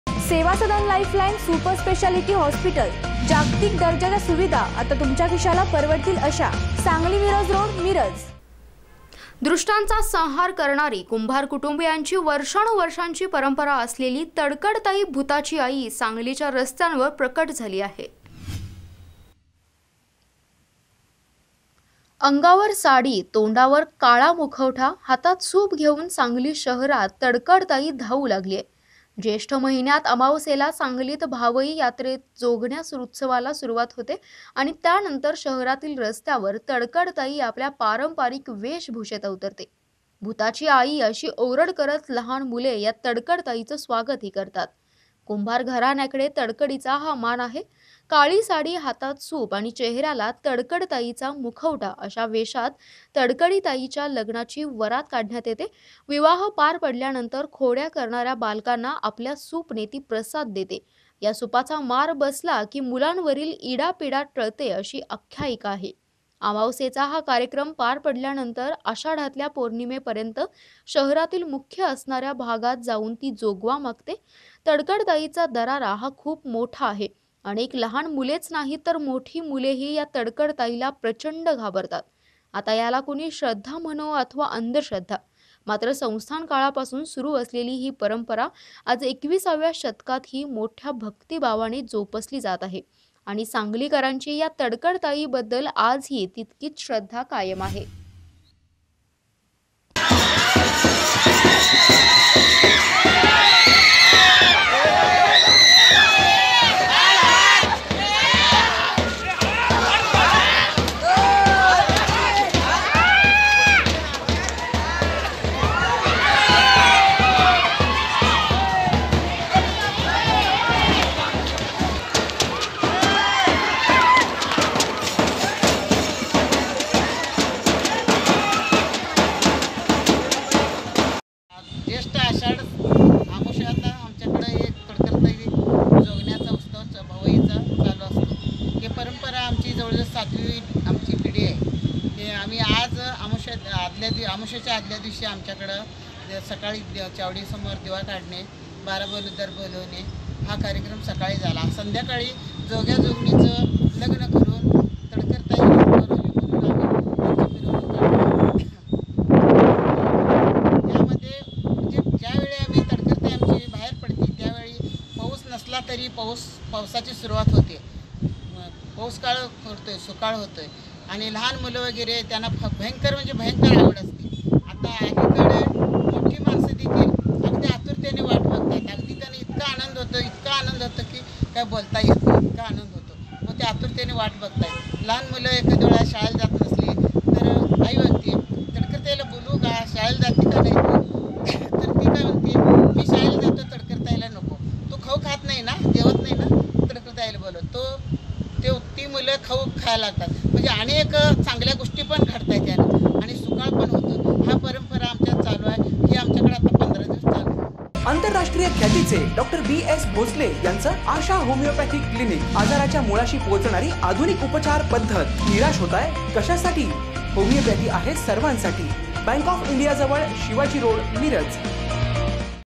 दृष्टांचा संहार करणारी 'तडकडताई भुताची आई' सांगलीच्या रस्त्यांवर प्रकट, कुंभार कुटुंबीयांची
दृष्टांचा संहार करणारी 'तडकडताई भुताची आई' सांगलीच्या रस्त्यांवर प्रकट, कुंभार कुटुंबीयांची वर्षानुवर्षे परंपरा.@